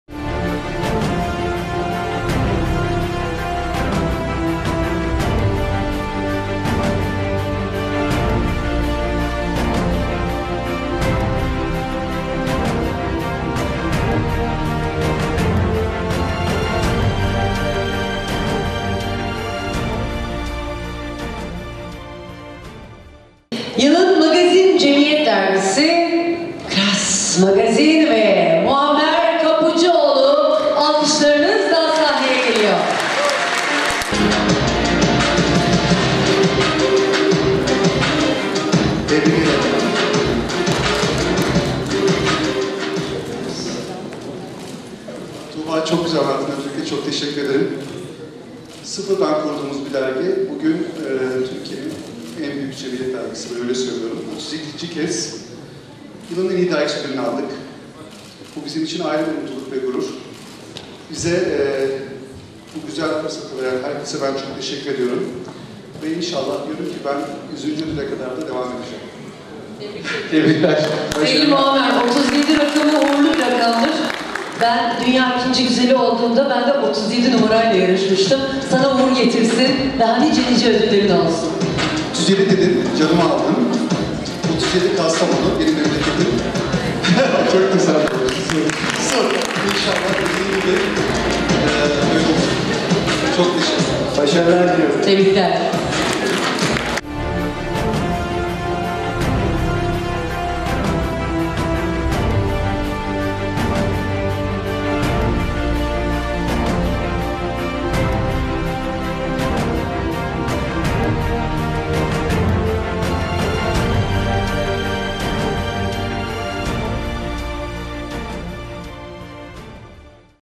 Klass Magazin Yılın Magazin Cemiyet Dergisi
Altın Rehber, Türkiye'nin önde gelen seçkin firma ve iş adamlarının yanı sıra; siyaset, magazin ve sanat dünyasından birçok kişi ver kurumların katıldığı Altın Rehber TV Yılın Enleri Ödülleri  2. etabını Fulya Sanat Merkezi'nde başarılı bir organizasyonla gerçekleştirdi.

Sunuculuğunu Murat PARASAYAR ve Tuğba ÖZAY'ın yaptığı gecede;
Yılın Magazin Cemiyet Dergisi KLASS MAGAZİN oldu.